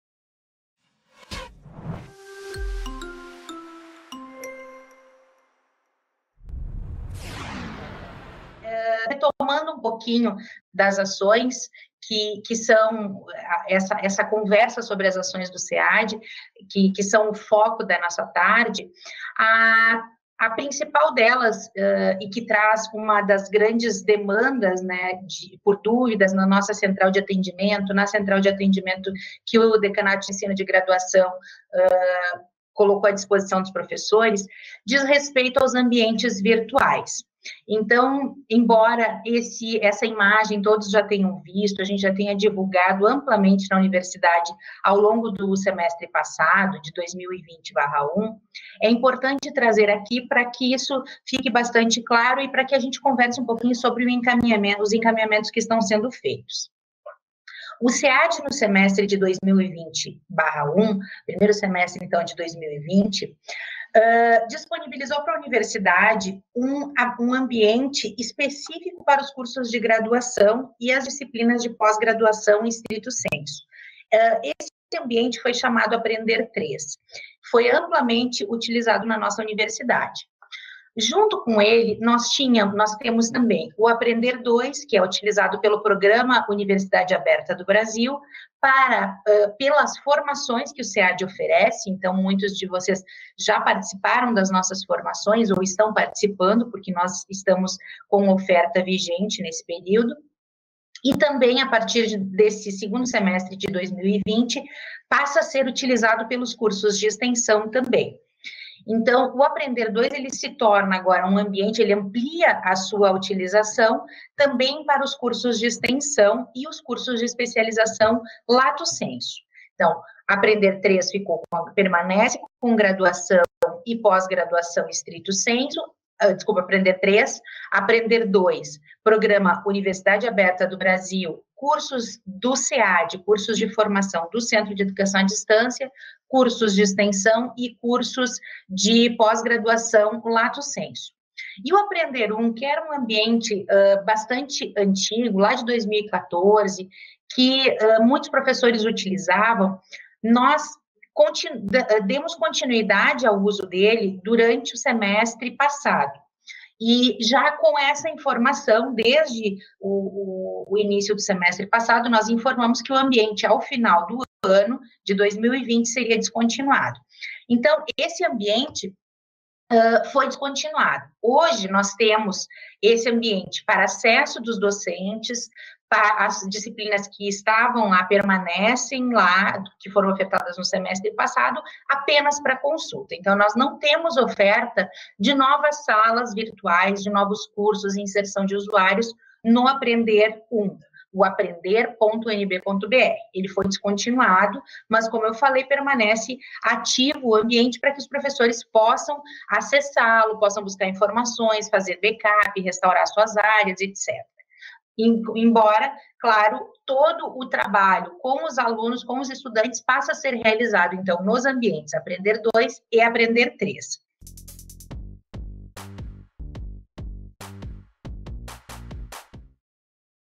Ambientes de Moodle UnB Aprender 1 2 e 3 (2021)
Você sabia que a Universidade de Brasília possui três plataformas de ensino diferentes? Conheça as diferenças entre o Aprender, o Aprender 2 e o Aprender 3, neste vídeo.
Se você quiser saber mais sobre o assunto, assista a esta live Conheça nossos tutoriais e ferramentas, acesse